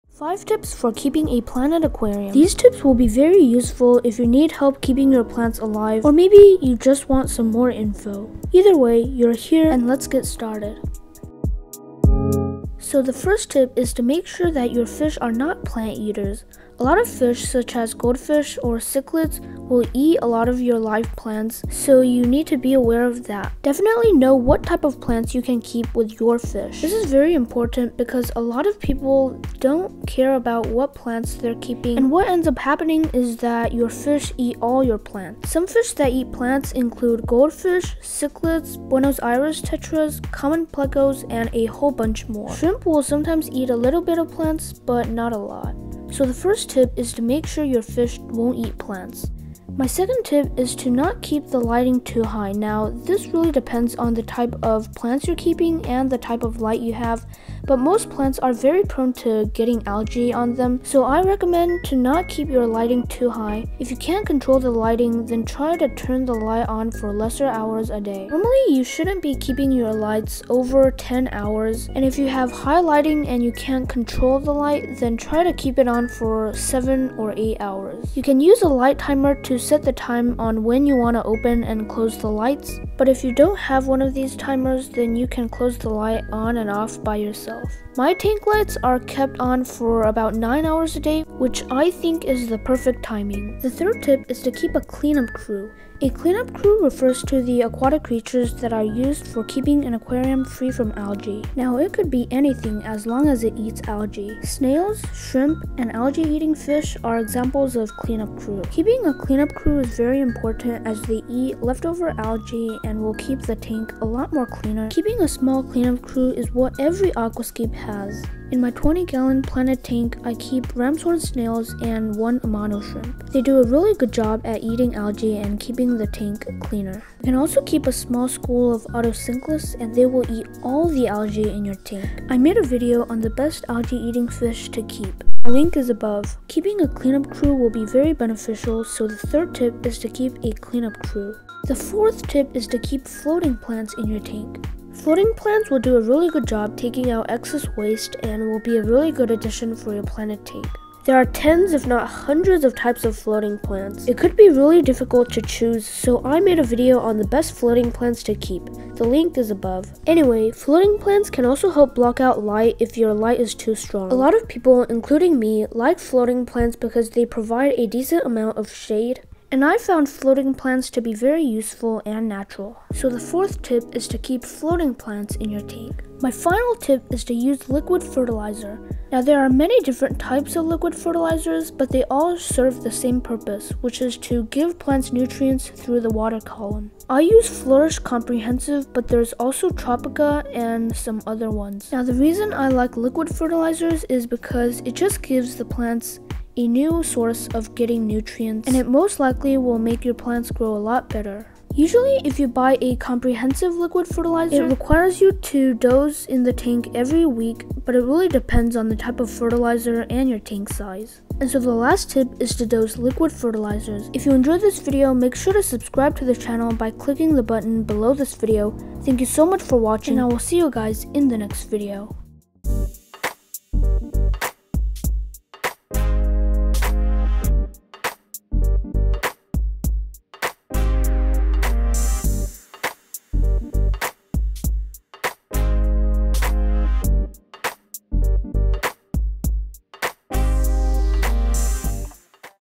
Top 5 BASIC Planted Aquarium Tips EVERYONE Should Know!
In today’s video, I’m going to give the Top 5 planted aquarium tips. In my experience, these tips will be great if you are looking to improve your planted tank. The tips will be ranked from least to best.

TIMESTAMPS:
0:00 - INTRO
0:13 - Tip #1
0:51 - Tip #2
1:35 - Tip #3
2:23 - Tip #4
2:57 - Tip #5